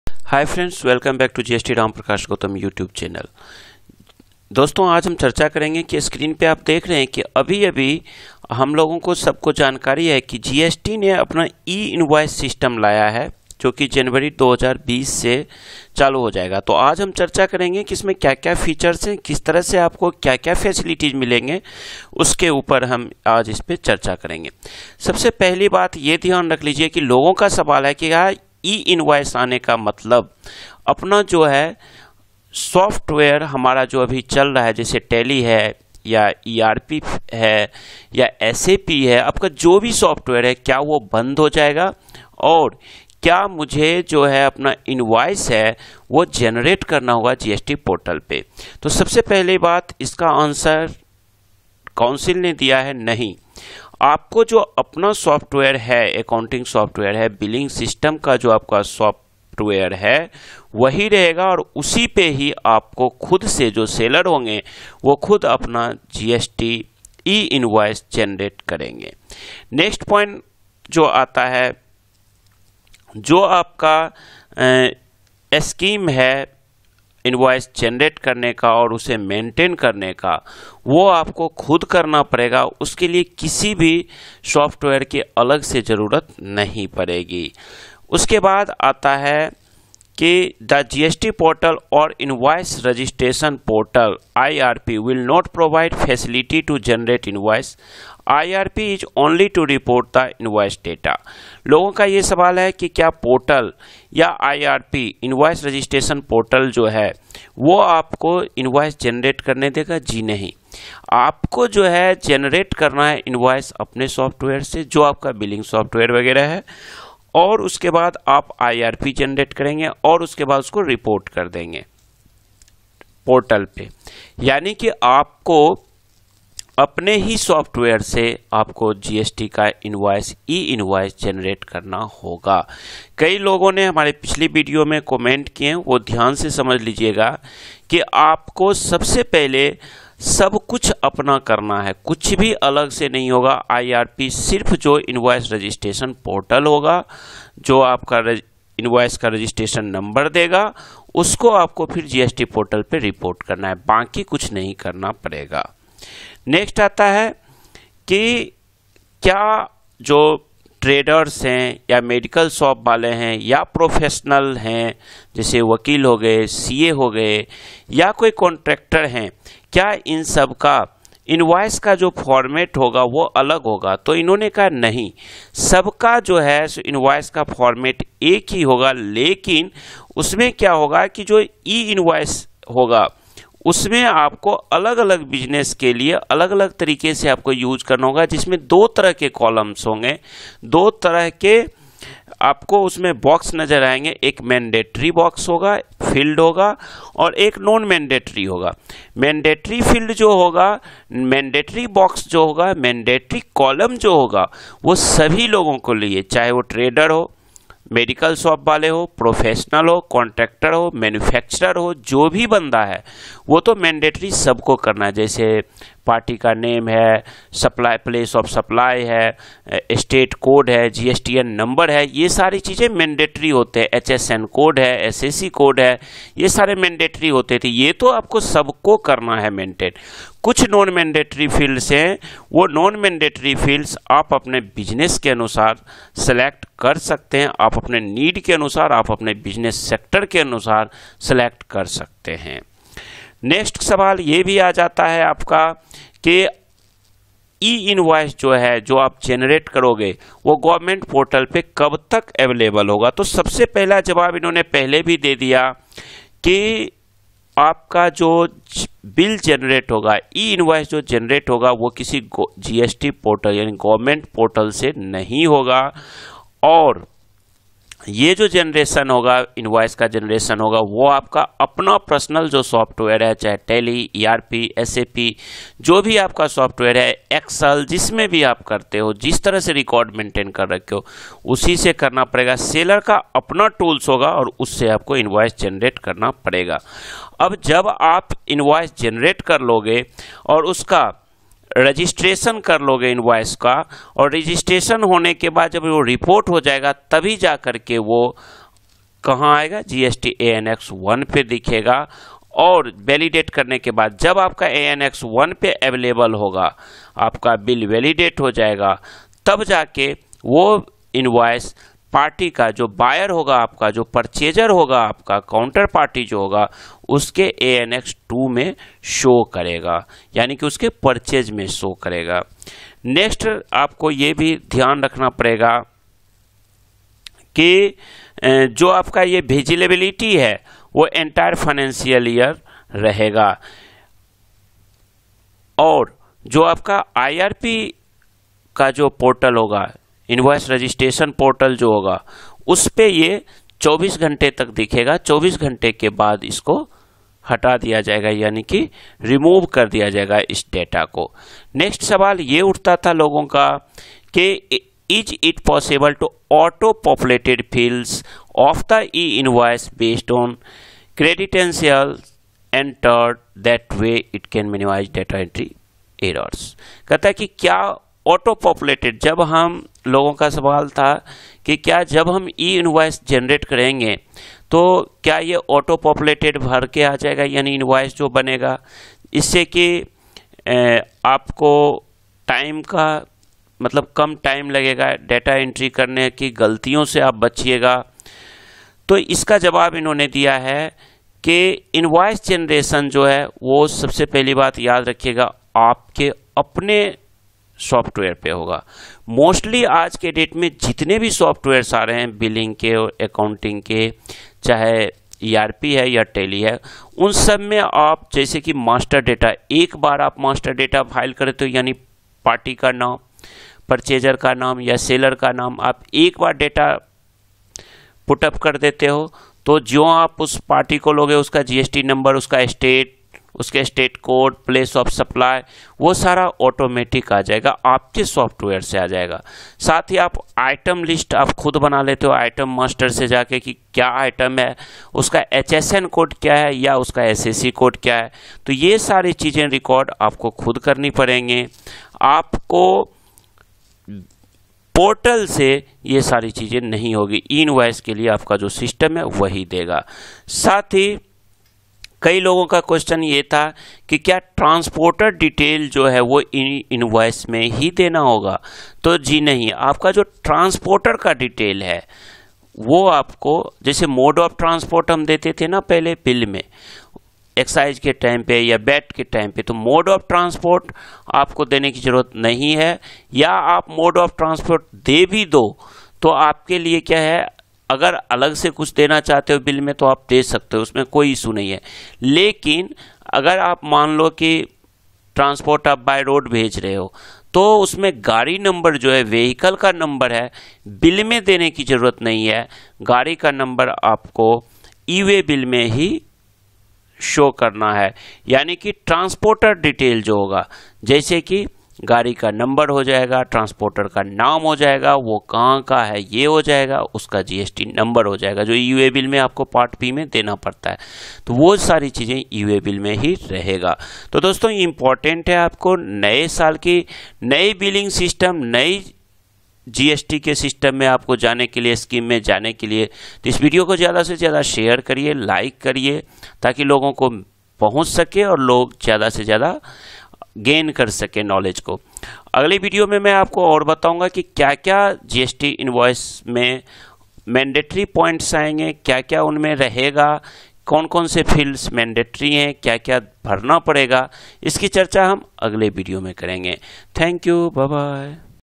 CLARIFICATION ON GST E INVOICE APPLICABLE FROM JANUARY 2020 || GST E INVOICE PROCEDURE
In this video we will discuss about the clarification issued by gst portal about gst E- Invoices. What is GST E Invoices and how to issue the E-Invoices in GST.